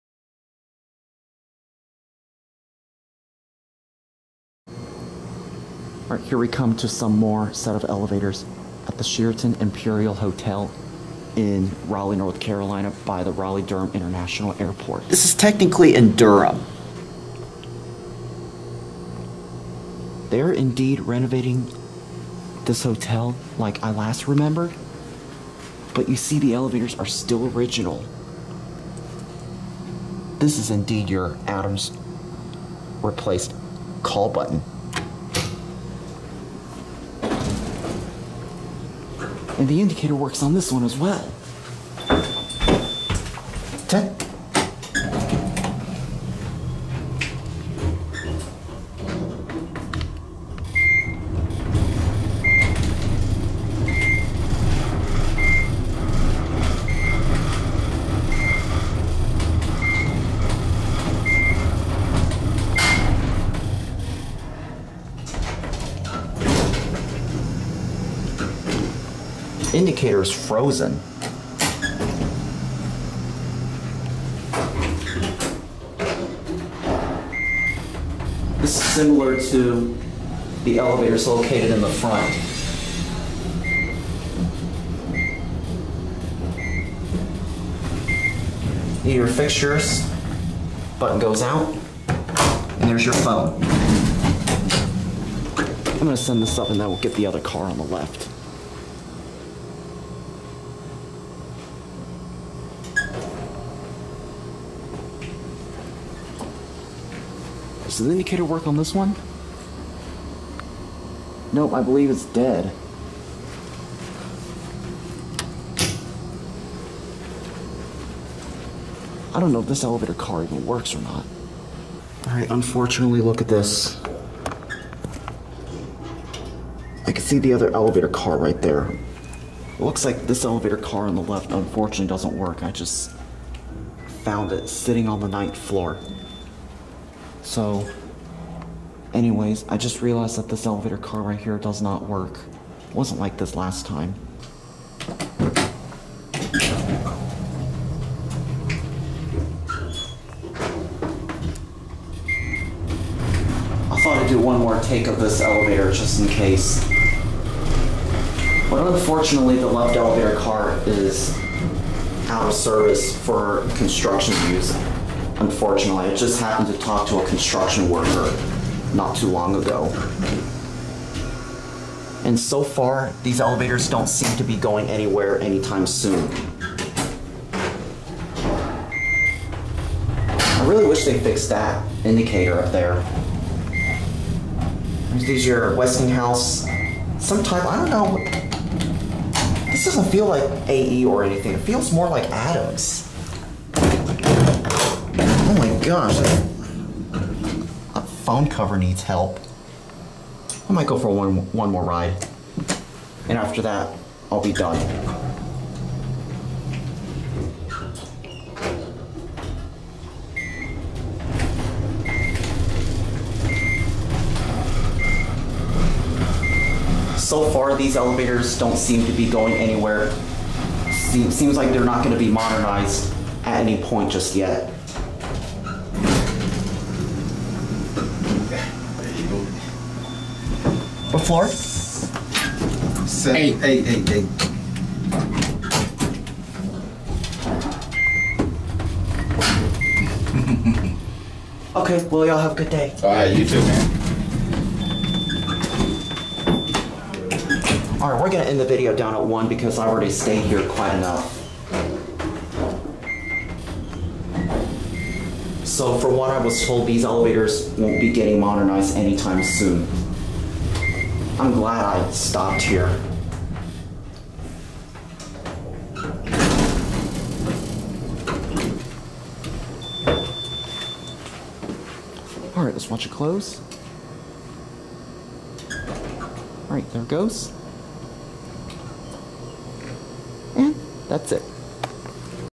Westinghouse Traction Back Elevators at Sheraton Imperial Hotel in Durham, NC.
Video Recorded on June 6, 2022: These are the second set which is tuck in the back behind the front elevators located in the main lobby and these are exactly the same as well also. Also, it looks like the floor indicator is stuck frozen on this elevator right here. I wish the left elevator car worked. For now, a construction worker told me that these elevators will not be modernized anytime soon unless the hotel decides to do so while renovating the hotel. Installed in 1986.